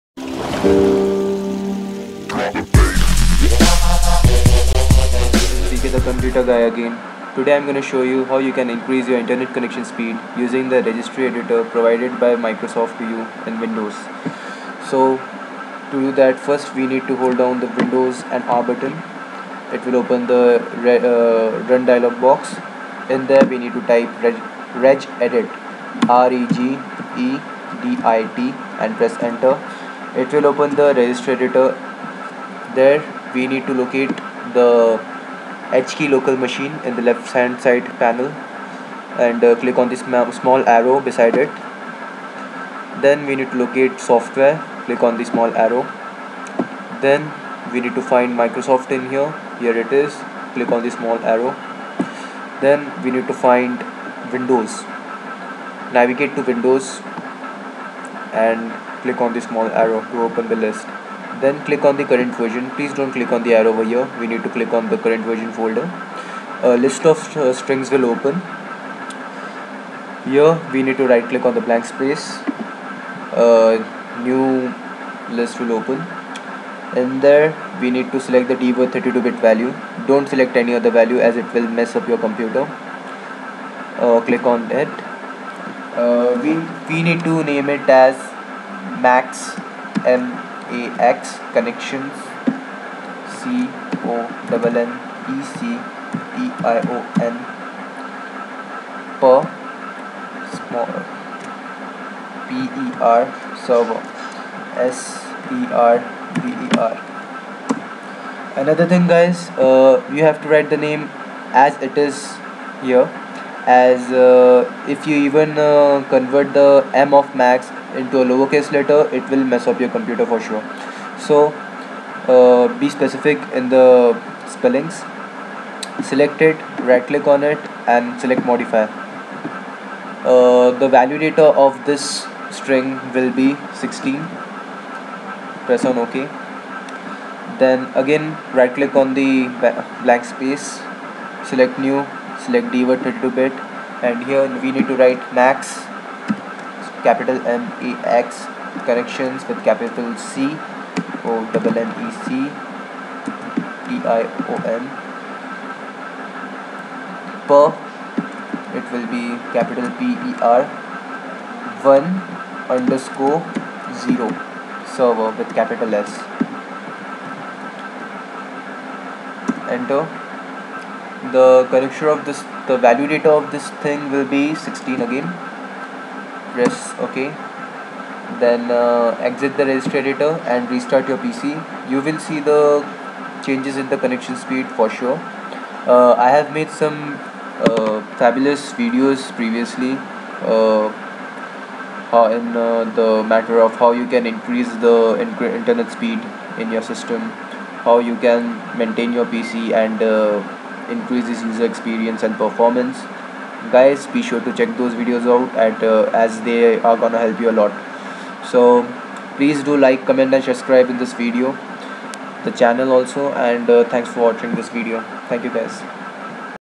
how to increse internet speed using registry editor
increase your internet speed drastically using registry editor